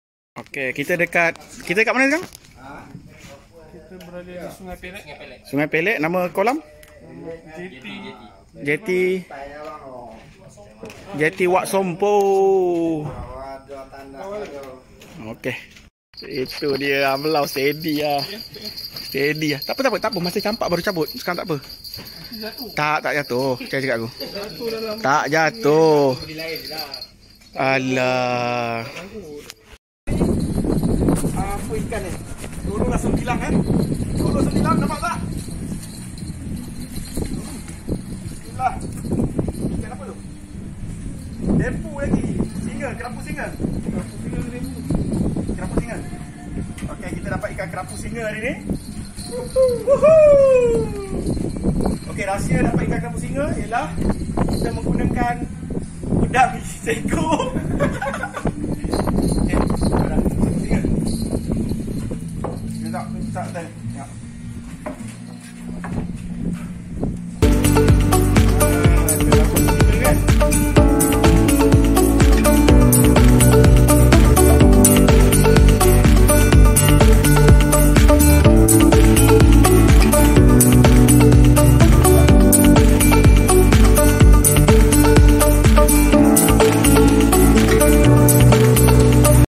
STRIKE Ikan Kerapu Harimau Memancing di Jeti Wak Sempoh, Bagan Lalang Selangor
Boleh makan ke ikan ni?